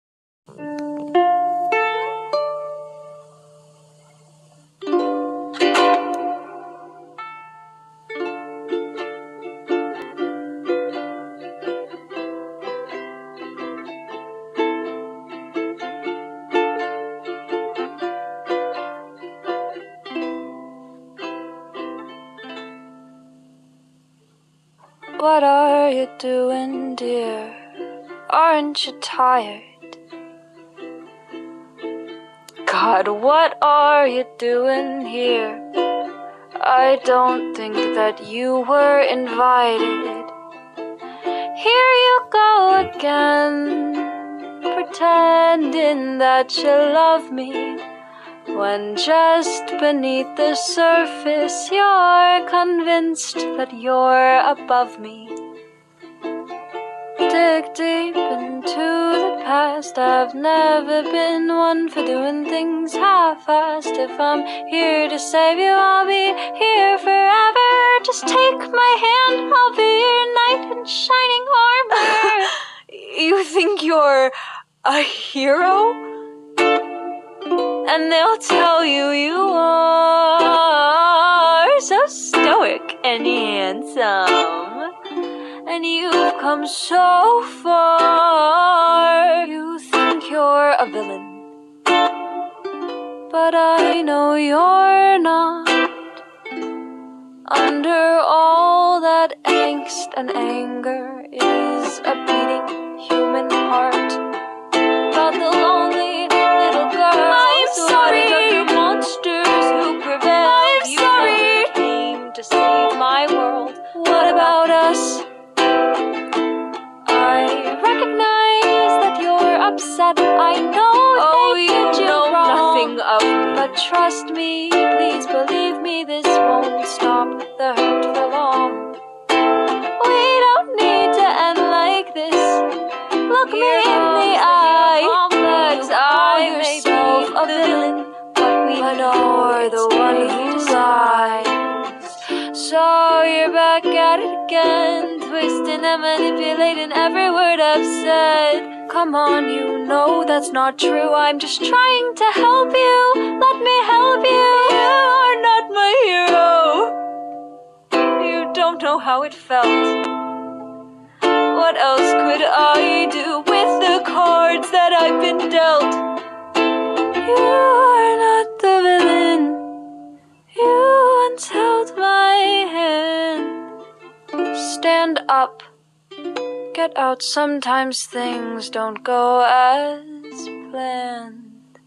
Trust Me Not (Kodi And Sammy)
So if you wanna know who Kodi and Sammy are Kodi is me IRL and Sammy is me from the past when I was 5 to be exact Kodi has told Sammy this but she refuses to belive that so Kodi has felt what Sammy has and more